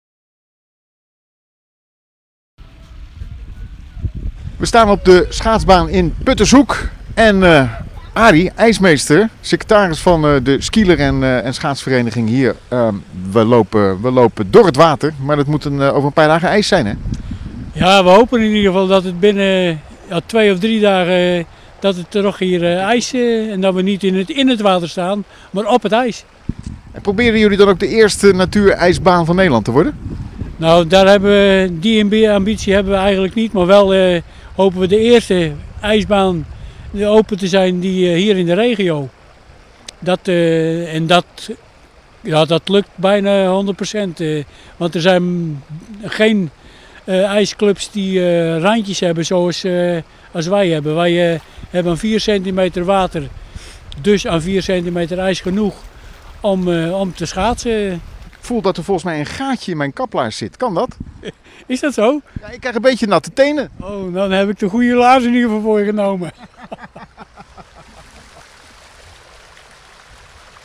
Videoverslag Kees Verkerkbaan Puttershoek
In Puttershoek zijn de waterpompen weer in gang gezet. De ijsmeesters van de plaatselijke schaatsbaan hopen de komende nachten genoeg vorst te krijgen zodat er snel op natuurijs geschaatst kan worden.IJsmeester en secretaris Arie van der Graaf van de IJs en skeelerclub Puttershoek houdt de weerberichten van Rijnmond-weerman Ed Aldus goed in de gaten. In het clubhuis staat permanent TV Rijnmond aan om de verwachtingen van Ed in de peiling te houden. De weersverwachting wordt zelfs via een beamer groot geprojecteerd op een scherm.Een opstaand randje&quot;We hadden een paar weken terug bijna alles voor elkaar maar toen sloeg de dooi toe&quot;, zegt de krasse ijsmeester op zijn watervaste kaplaarzen. Terwijl hij door het snelstromende water banjert vertelt hij dat de baan afgelopen jaar is gerenoveerd.&quot;We hebben nu een 200 en een 400 meter baan zodat er ook geshorttrackt kan worden&quot;, zegt Van der Graaf. &quot;We hebben ook met hulp van de gemeente een opstaand randje laten maken zodat we minder water nodig hebben voor een mooie ijsvloer&quot;. Weerman Ed AldusWanneer kan er dan geschaatst worden op natuurijs in het geboortedorp van Kees Verkerk? &quot;Ik ga dat nog niet zeggen, eerst maar eens de komende nachten afwachten, en kijken wat de wind doet&quot;, zegt de ijsmeester voorzichtig.Met een beetje hulp van weerman Ed Aldus zou het mogelijk begin volgende week kunnen worden, mits, maar, tenzij, enfin: aan ijsmeester Arie van der Graaf en zijn mannen zal het niet liggen. De krasse knar lacht zich een deuk als verslaggever Ronald van Oudheusden meldt dat er toch echt een gat in zijn geleende linker kaplaars zit. &quot;Dan heb ik je tóch de goede gegeven&quot;, lacht hij. &quot;Maar als we open gaan ben je van harte welkom&quot;, en dat geldt uiteraard voor een ieder.